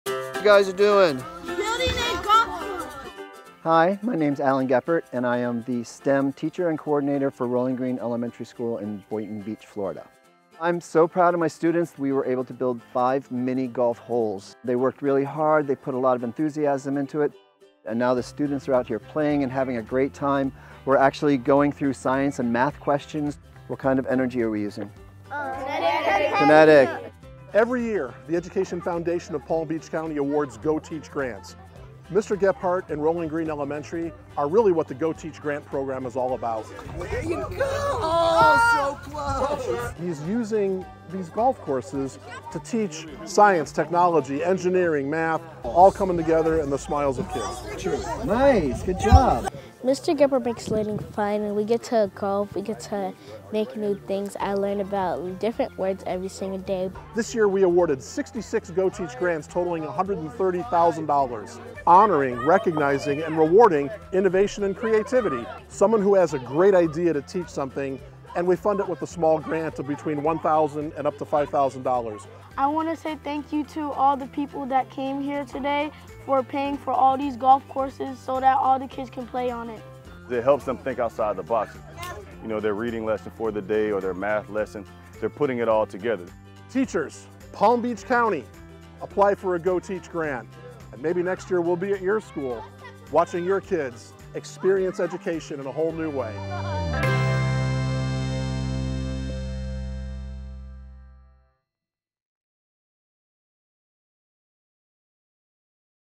Students Build Mini Golf Course on School Campus
Students at Rolling Green Elementary get to practice their putting skills while learning about science, technology, engineering, and math. Alan Geppert and his students designed and built a mini golf course on campus with a GoTeach! Classroom Grant from the Education Foundation of Palm Beach County.